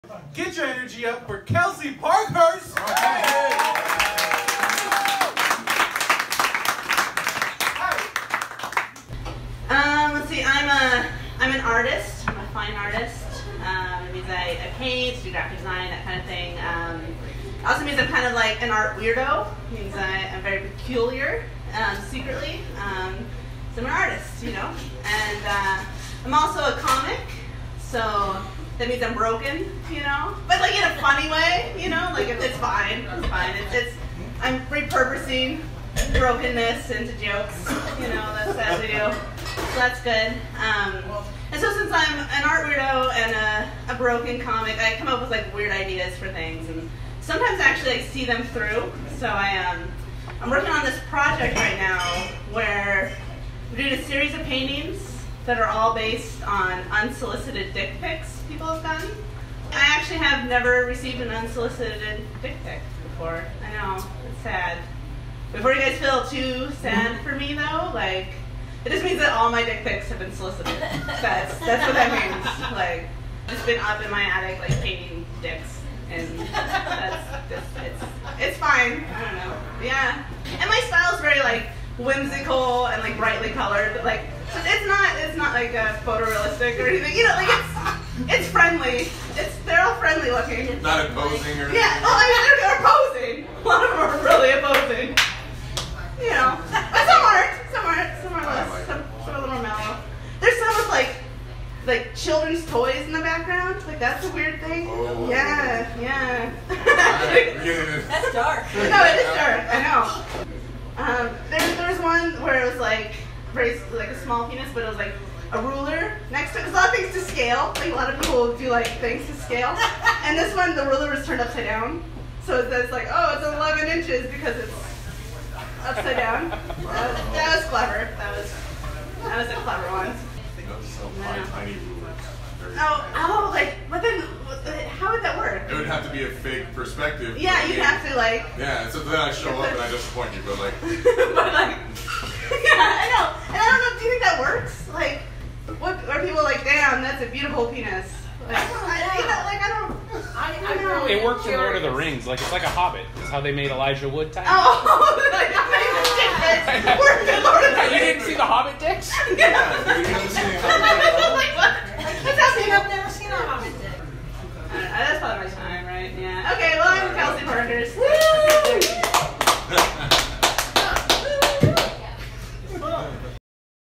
Unsolicited - Comedy Show Trailer for 2/22/20
Come hear rad comics: Jenna Vesper, Courtney Byrd, Tambre Massman, Stéphanie Nam, Kelsey Parkhurst, and Kelly Wilson share jokes about their experiences with unsolicited dick pics. A pop-up gallery chalk full of Kelsey's Unsolicited paintings will be on display.

Come see us in Tacoma, at Fern & Foster Feb 22nd at 8pm, or paintings only Feb 20th 6-8.

From the artist: "For years now, I’ve thought about the canon of fine art, and how it mainly followed the model of “white male artist with a nude female (often faceless) subject.” I had an idea to recreate classic paintings, but gender swap them. Men lounging around naked, taking baths, a cubist dick, etc. But I am not a classical, style painter. My work is more abstract, whimsical, and brightly colored. I couldn’t pull it off.

Thinking about desire, consumption, gender, cultural norms; I had another idea. I wanted to do a painting series based off of unsolicited dick pics. The unsolicited dick pic, is such a cultural thing, but the context behind each pic is so varied. Unsolicited dick pics are at the apex of sex, power, technology, rape culture, vulnerability, and are also pretty freakin’ funny.

This project has expanded to include a series of paintings, a documentary, and a comedy show (Feb 22nd). The process of making these paintings has been funny, sexy, oppressive, overwhelming, and strange."